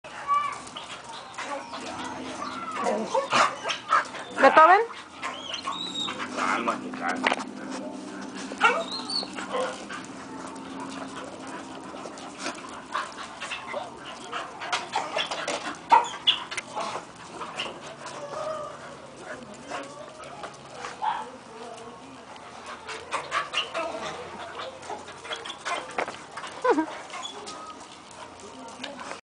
Bethoven2.MPG
Bethoven(2) es un perro de aproximadamente 1 año 6 meses de edad, color negro con café (atigrado), pelo corto y tamaño grande. Fue rescatado junto con su hermano vagando por la calle en un fraccionamiento. Ya está desparasitado, vacunado y esterilizado. Es un perro dócil, sociable y cariñoso. Le gustan los juguetes y es sociable con otros perros. Necesita un lugar amplio en donde hacer ejercicio, así como una familia responsable y cariñosa. Si te interesa adoptarla, puedes comunicarte a través de vía correo electrónico a o al teléfono (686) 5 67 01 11 de lunes a sábado entre 9:00 a.m. y 2:00 p.m.

Video: 2 de enero de 2011